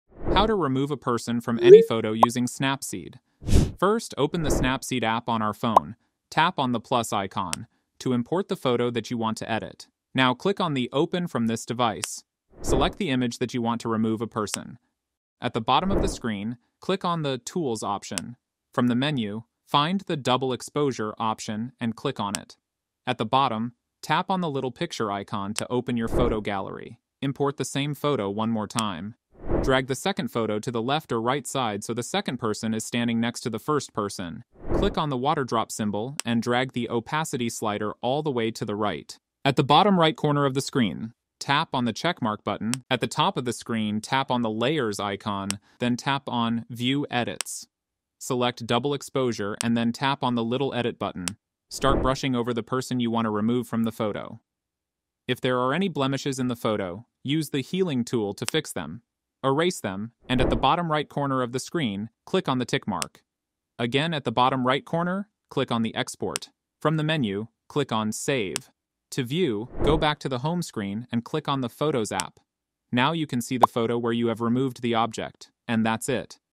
How to Remove a Person from any photo using Snapseed?
In this step-by-step tutorial, learn how to effortlessly remove anyone from a photo using Snapseed! Whether you're looking to enhance your images or simply want to eliminate distractions, this video will guide you through the process of using Snapseed's powerful editing tools to achieve stunning results in just seconds.

We'll cover:
- The basics of Snapseed
- How to use the Healing tool effectively
- Tips for seamless photo editing
- Tricks to maintain the quality of your images

Don’t let unwanted people ruin your perfect shot! Follow along as we demonstrate the techniques you need to create a cleaner, more focused image. Perfect for beginners and experienced users alike, you'll be amazed at how easy it is to transform your photos.